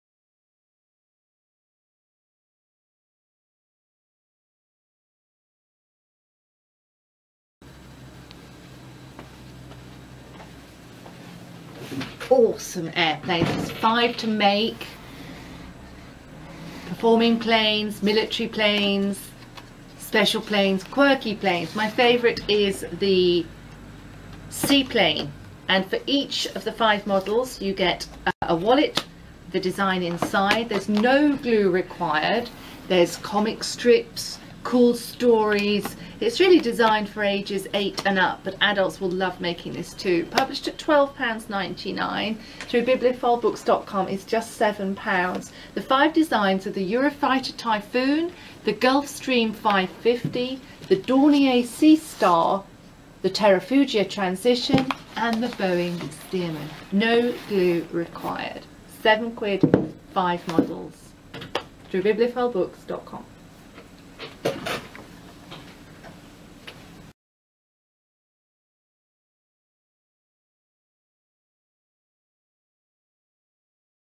AWESOME AIRPLANES MODEL BOOK
DAVID HAWCOCK ET AL    Book Number: 75323    Product format: Paperback

This unusual offering is sure to delight all young fans of aircraft - and their dads and granddads! Avoiding commercial airliners, the book looks at five classes of aircraft: Private Planes (from microlights to the Gulfstream G550), Performing Planes (from Barnstormers to the Red Arrows), Military Planes (from the Red Baron to the Eurofighter and the US F-22 Raptor), Special Planes (Flying Doctor, crop-dusters and firefighters) and Quirky Planes (from the five-winged Fokker V8 to the Solar Challenger and the Terrafugia flying car). For each category, there is a history of that class of plane, colour and b/w photos and a super cartoon strip illuminating the story of one notable, and then, best of all, there is a make-it-yourself model. Simply slot together the pieces, no glue or paint required. The models are a Gulfstream G550, a 1940 Boeing Stearman Biplane, a Eurofighter Typhoon, a Dornier Seastar amphibious and the rather wonderful (I-want-one-now!) Terrafugia Transition flying car, surely the best commuter car ever. 10 thick card pages, 8¾"×10¼".